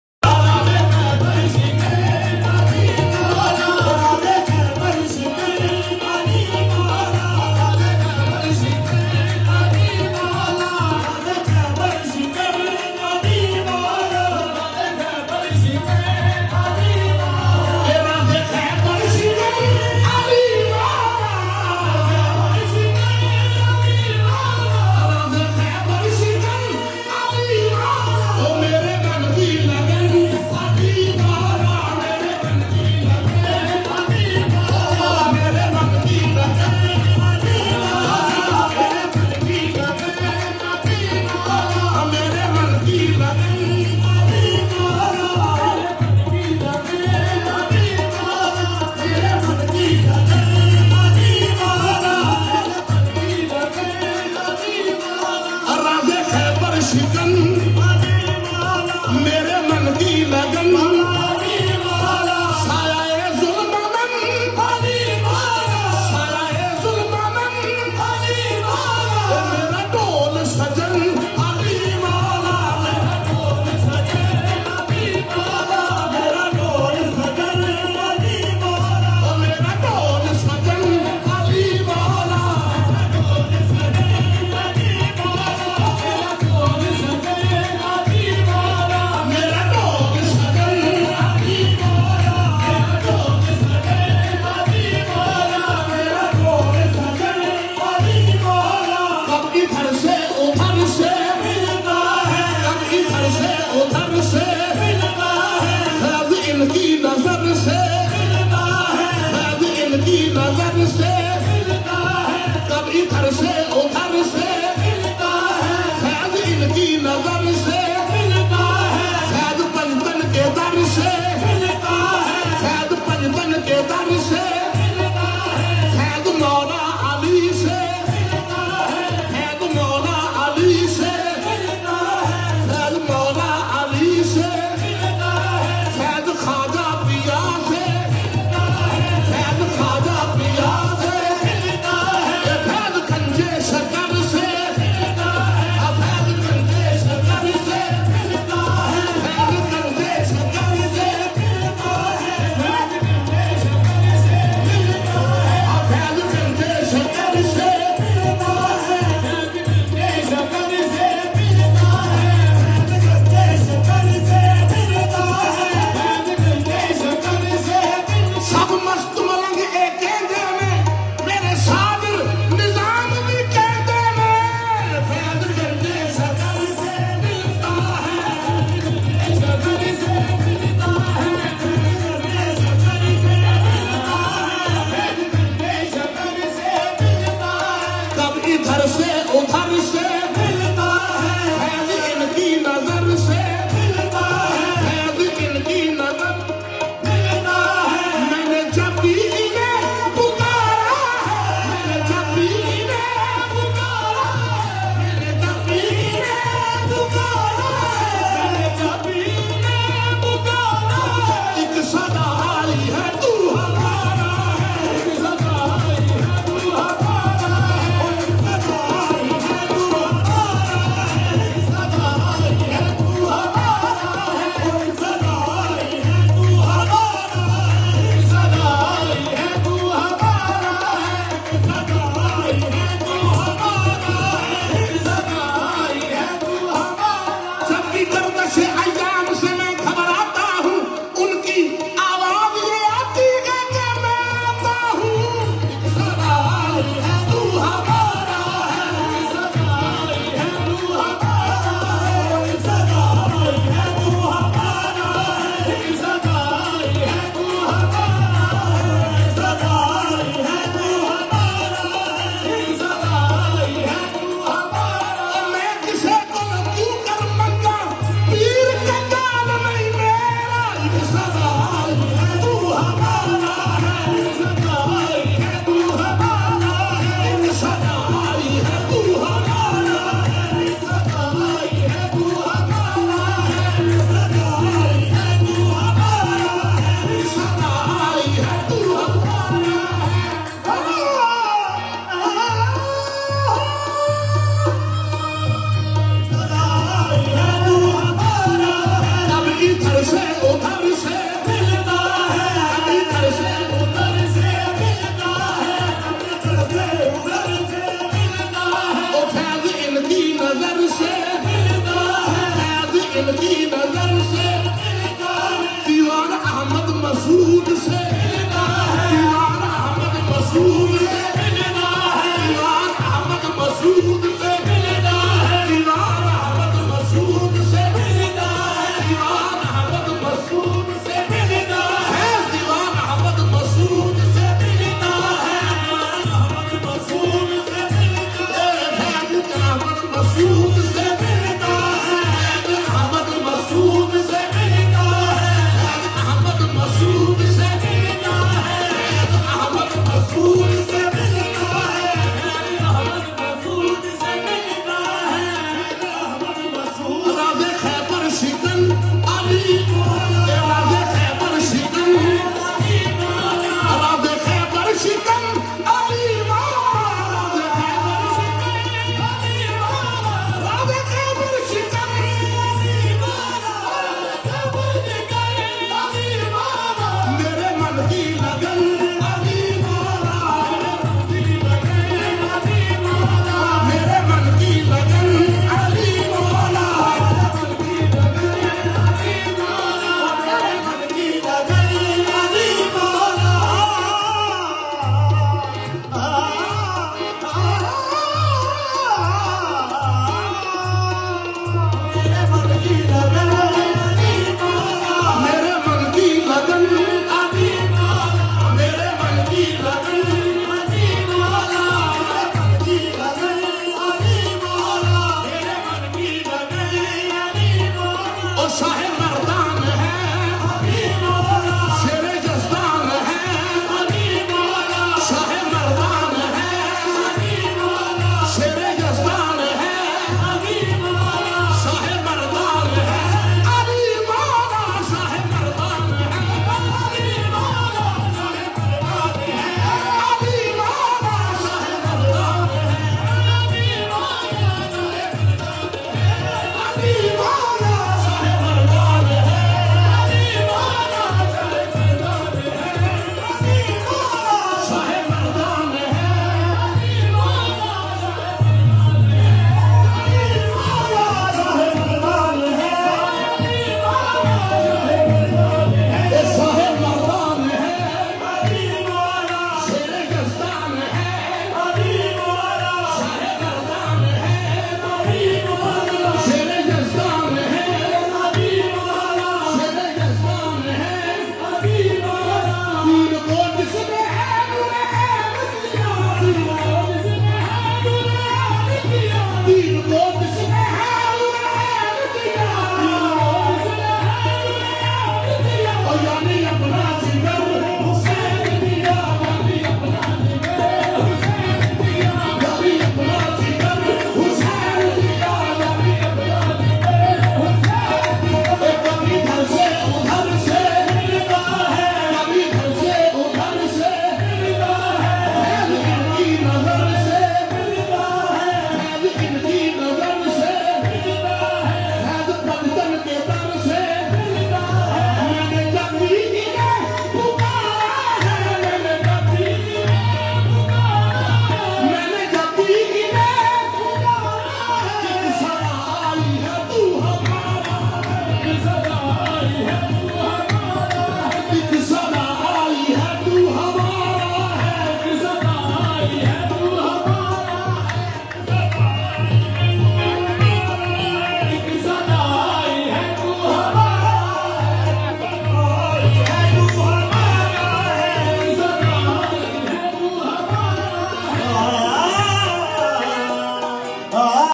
Uras Syed Manzoor Alahi Gadhi Nashen Peer Ameer Ali Qadri Darbar 2Chak Ramdewali Sargodha Road Fsd
1.1.2022.Uras Syed Manzor Alahi Gadhi Nashen Peer Ameer Ali Qadri Darbar   2Chak Ram Dewali Sargodha Road Fsd Zere Sadarat Janab Qibla Dewan Ahmed Masood Chishti Zaib Sajjada Baba Farid Ganj Shakar Pakpatan